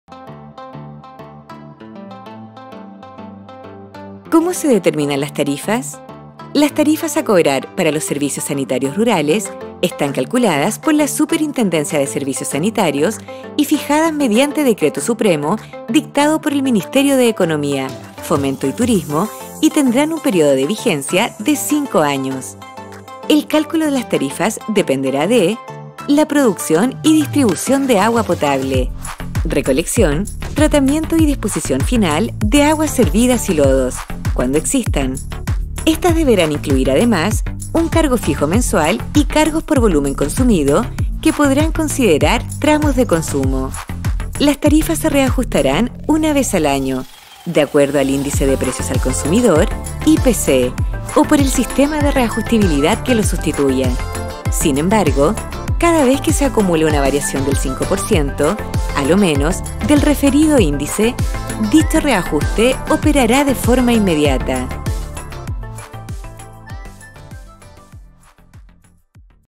MG. Servicios Sanitarios Rurales I.
MG realizado para la Superintendencia de Servicios Sanitarios que entrega información sobre los Servicios Sanitarios Rurales.